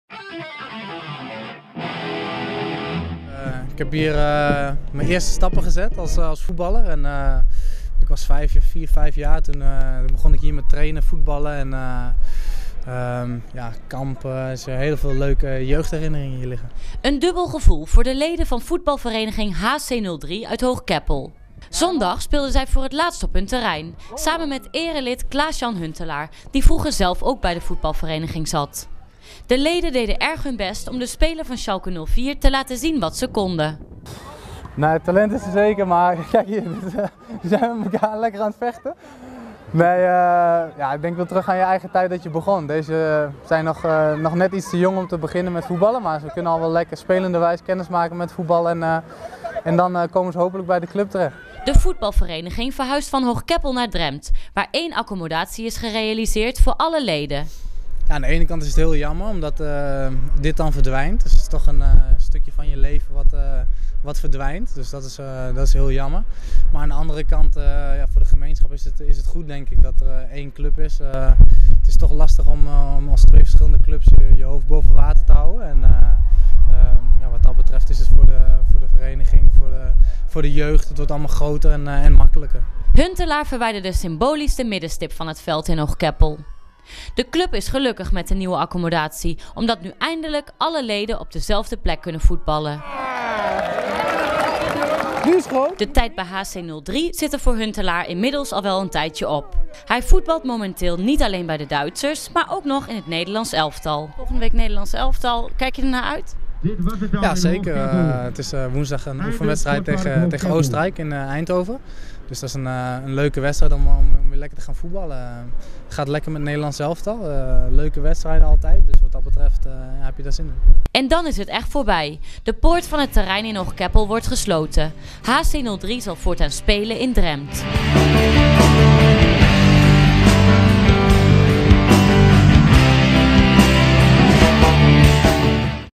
Klaas Jan Huntelaar op bezoek bij zijn eerste club HC'03
Voetballer Klaas Jan Huntelaar was op bezoek bij zijn oude club in Hoog Keppel